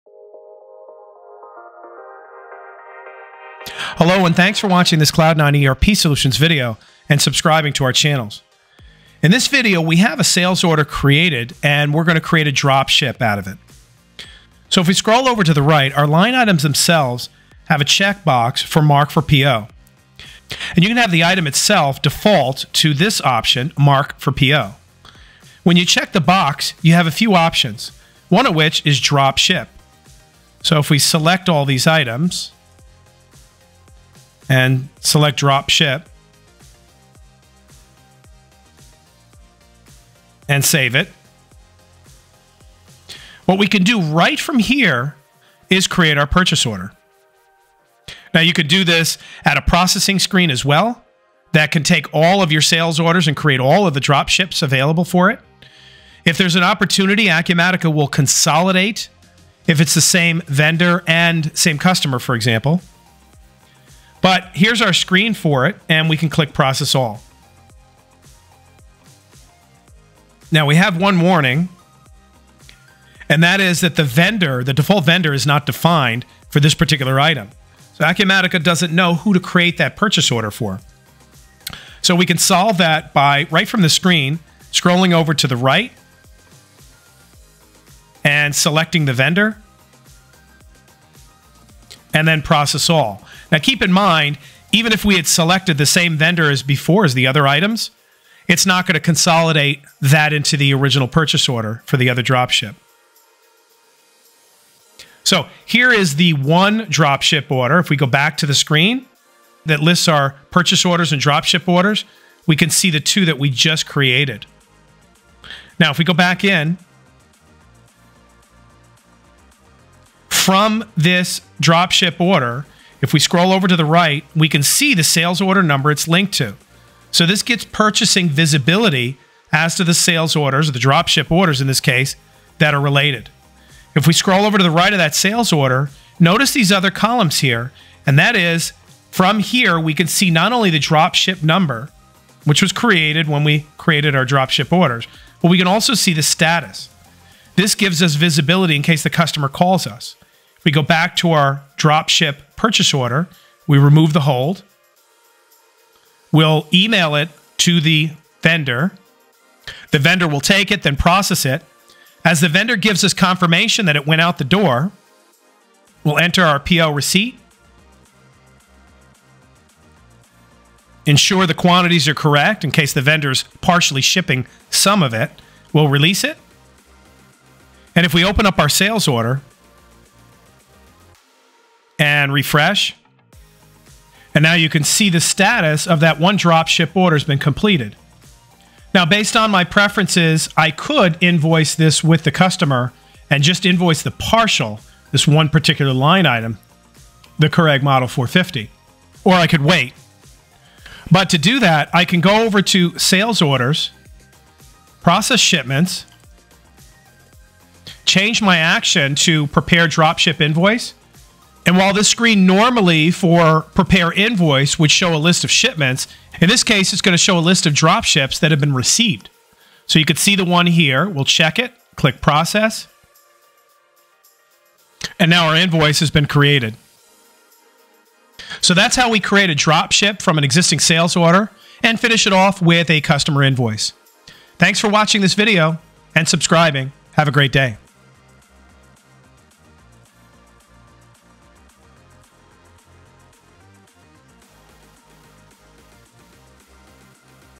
Create Drop Ship and complete with Acumatica
Learn how to easily create a drop ship from a sales order and track it easily and then invoice it.

With Acumatica whether you're sales, purchasing or finance you can easily track the status of the purchase order, sales order and never forget to invoice  your customers with drop ship sales orders.